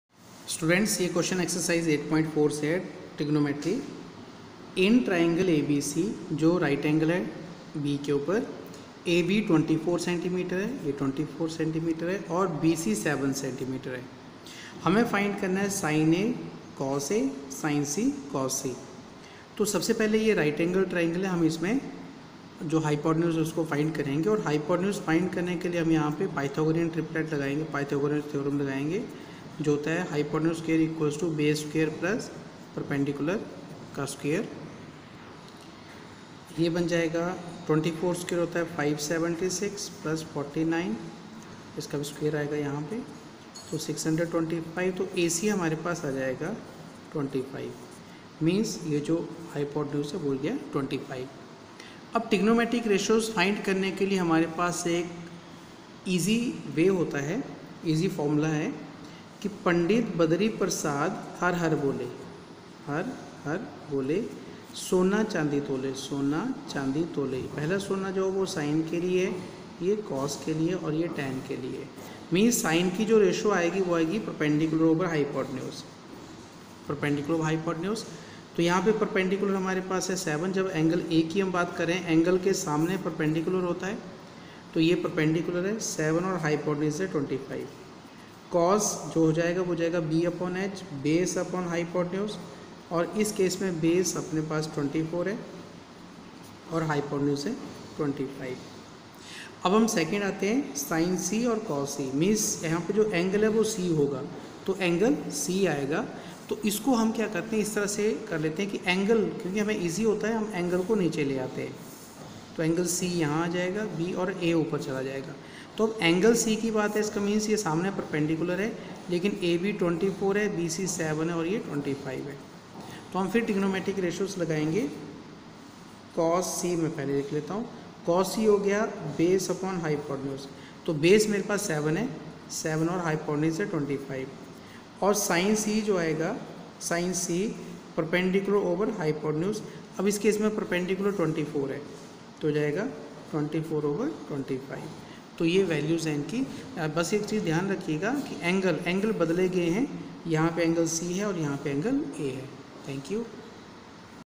Ex8.1, QNO1, In ∆ABC right angled at B, AB = 24 cm, BC = 7 cm. cos CCLASS 10, INTROD to TRIGONOMETRY
In ∆ABC right angled at B, AB = 24 cm, BC = 7 cm. Determine:
(i) sin A, cos A
(ii) sin C, cos C